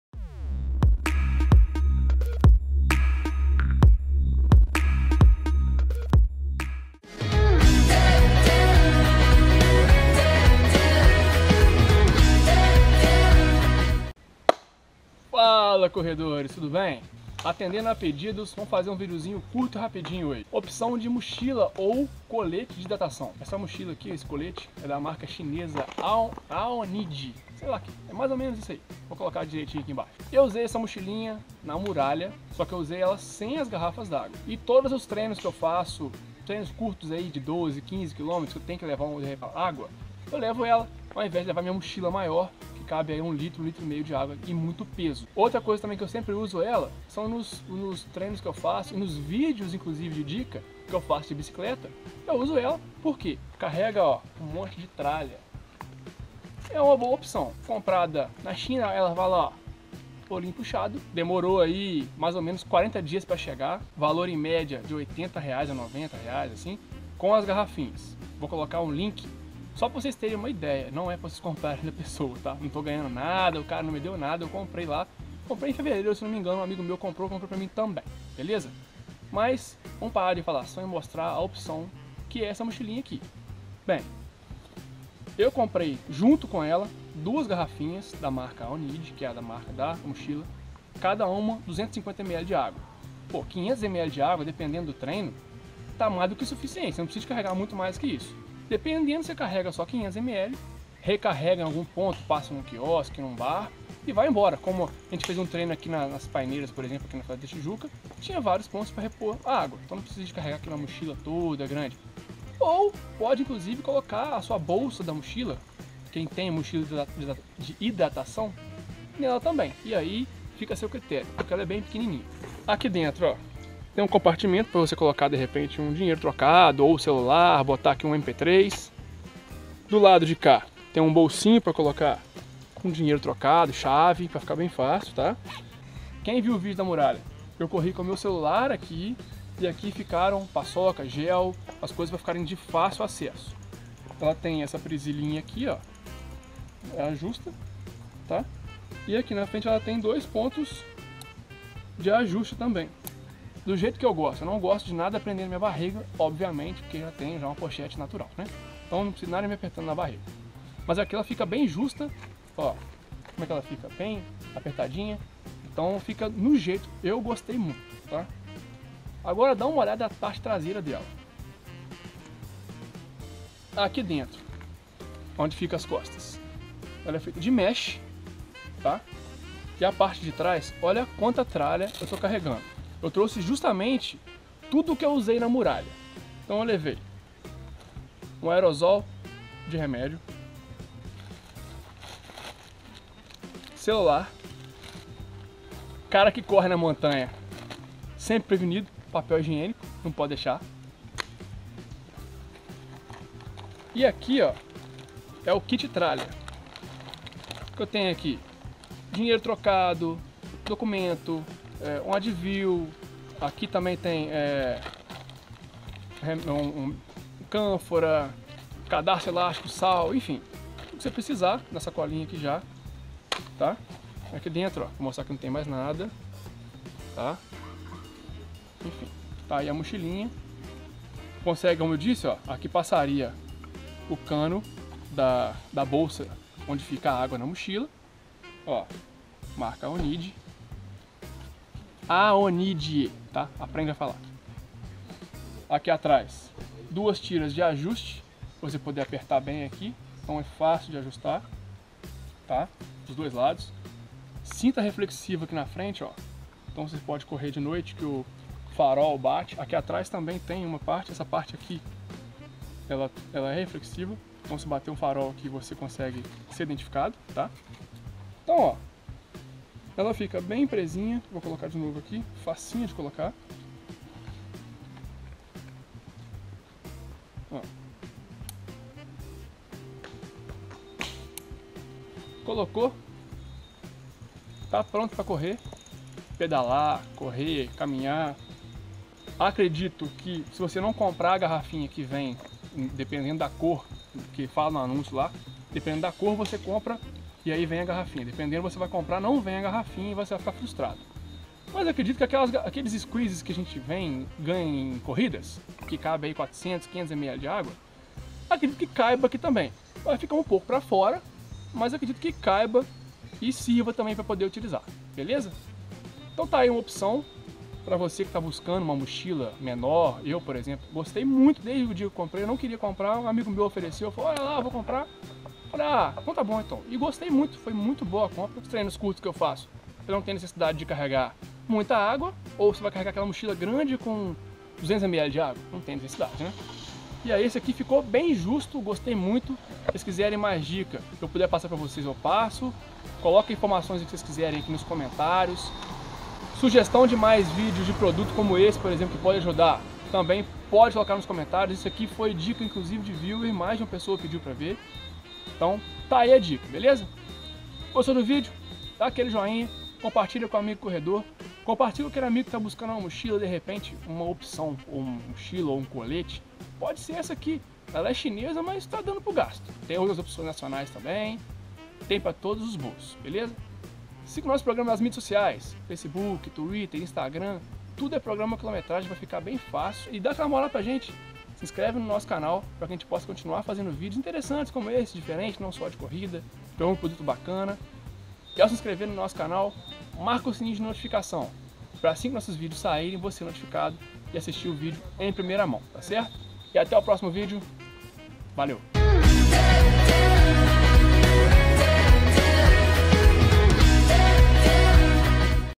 Review Mochila/colete de hidratação Aonijie
Faaaaaala corredores tudo bem? Você que busca uma mochila menor ou colete de hidratação para treinos ou provas de média distância, ou uma mochila mais leve, essa pode ser a solução. Apresentamos no vídeo de hoje a mochila/colete da Aonijie. Quer saber mais? Então assista o vídeo!!

- Link de exemplo da mochila